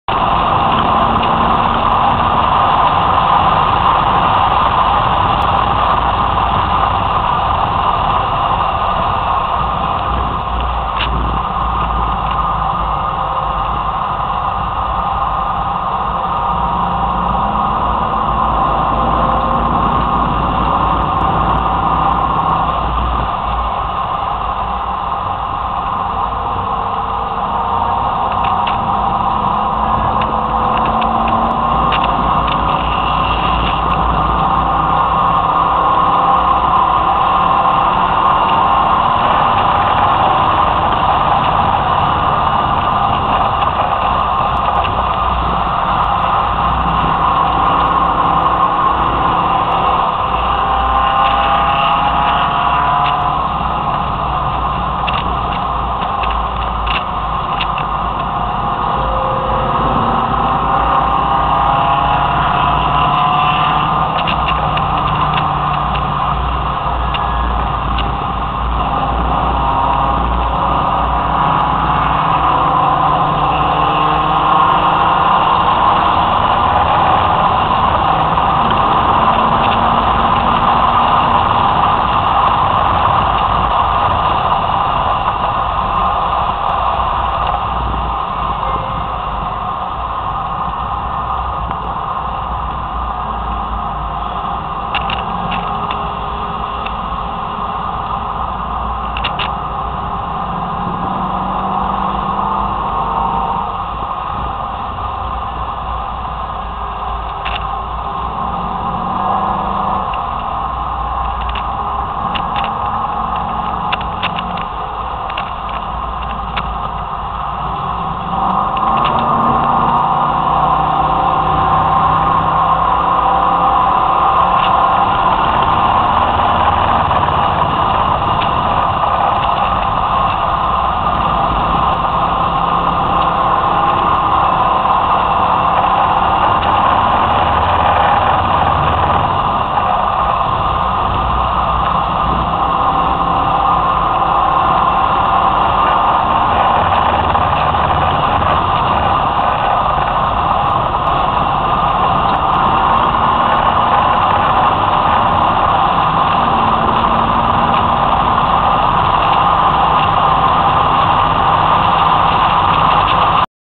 Lime Rock Park 09/20/2011 SCDA
Lime Rock Park 09/20/2011 SCDA  fourth run of the day a few laps. Car is 1989 Trans Am GTA